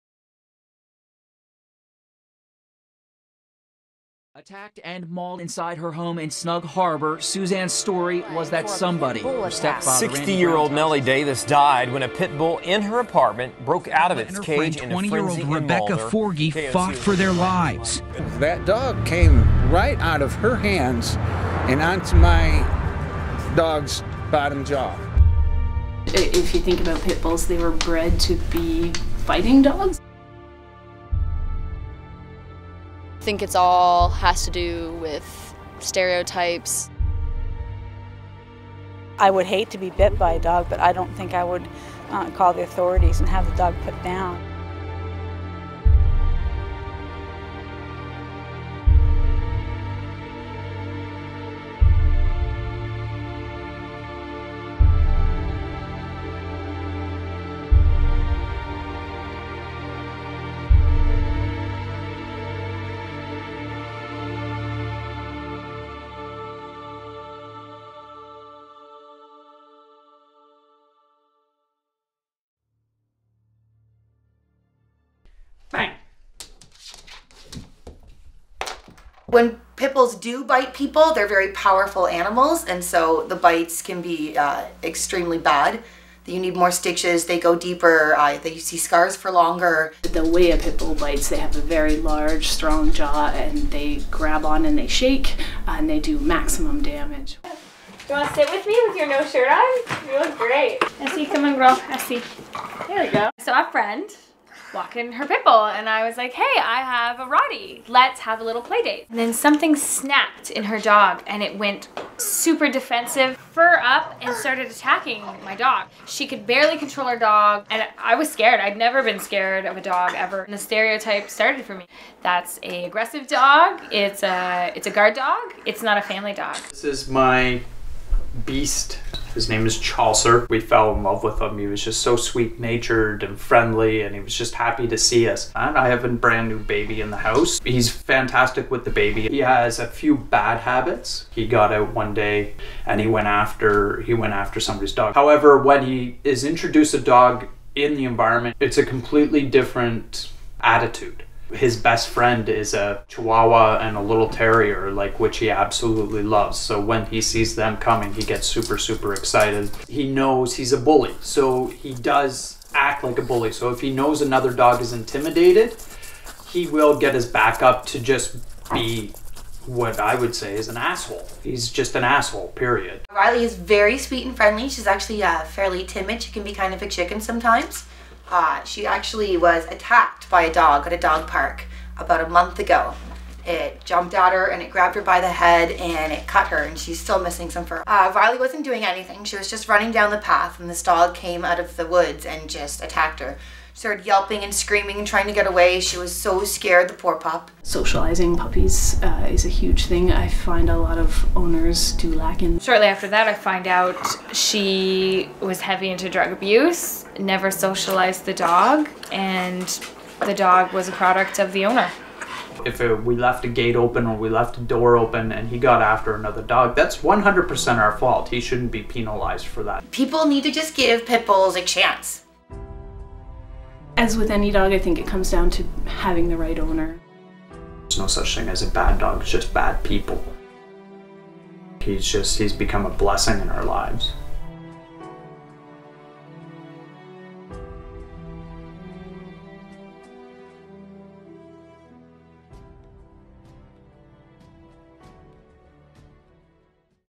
A Banned Breed - Documentary
A group of first year students in Algonquin TV Broadcasting produced a 4 minute documentary covering the mannerisms, and stories about Pitbulls.